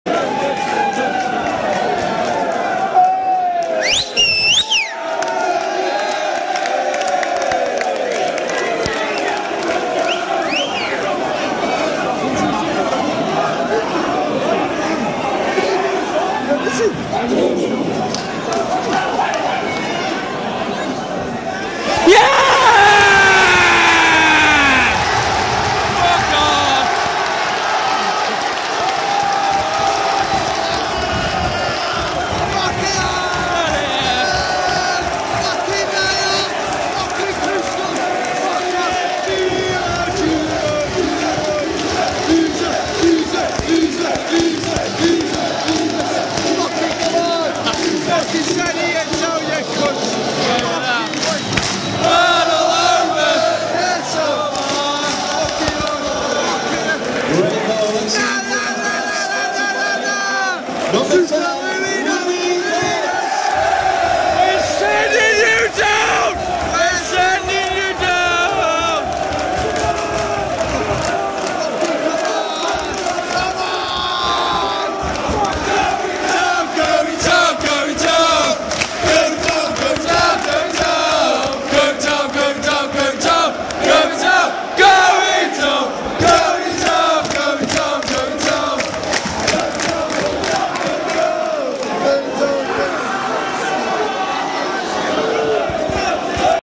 Port vale vs crewe Louis dodds 1st goal
Port vale 3-0 crewe Alex
Port vale relegate crewe Alex
Bye bye crewe
We are port vale and we're sending you down!